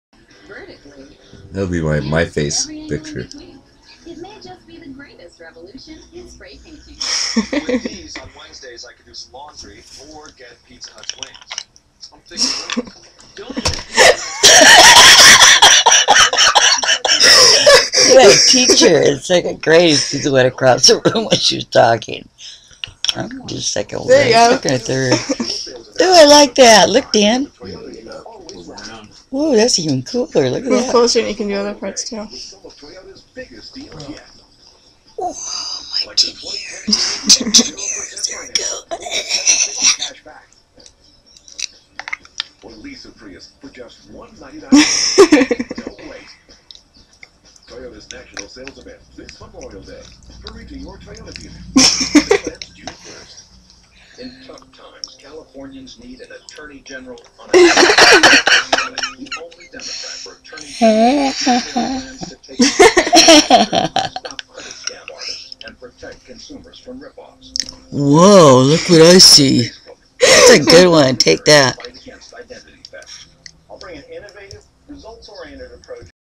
Denture twist
Showed my mom our new comps webcam features...yeah